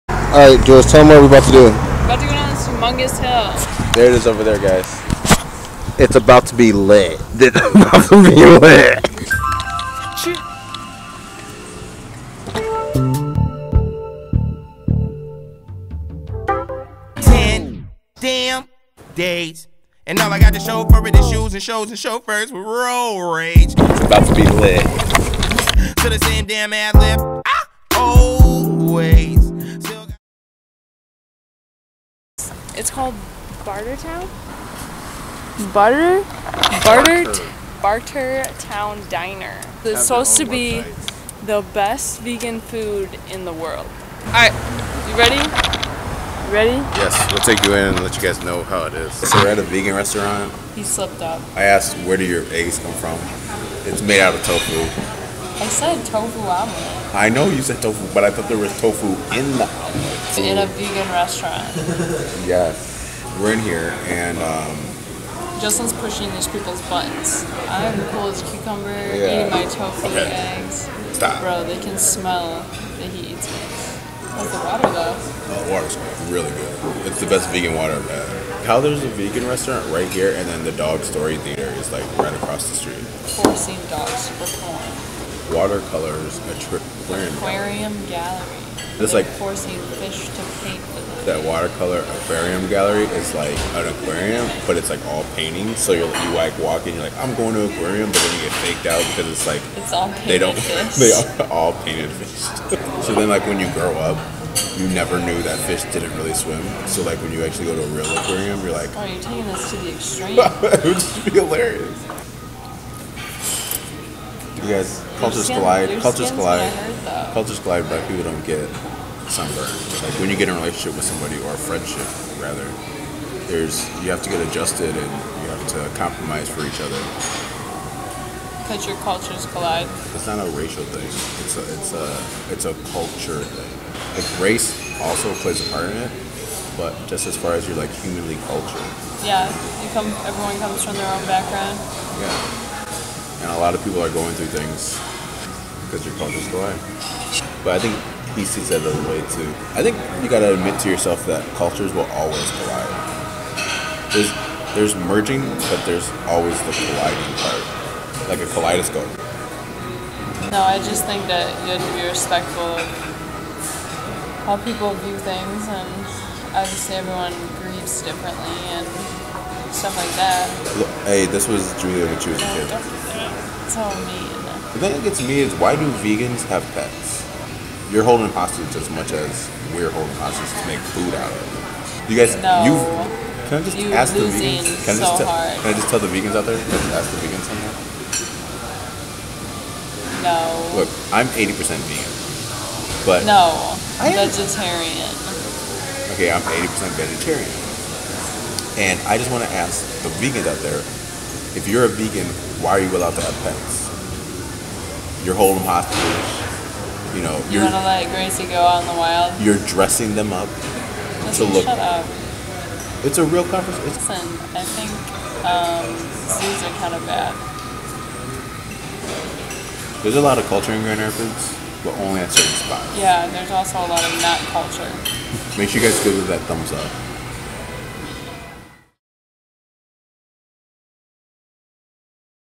EXTREME BIKING CHALLENGE!
Woahhhh! Extreme bike tricks here as well as asking all the wrong questions while at a vegan restaurant... Either way watch as we have a great time this week and hit that subscribe button so you don't miss out on Part too coming out this Sunday!

Bye guys! Thanks for all the love and support.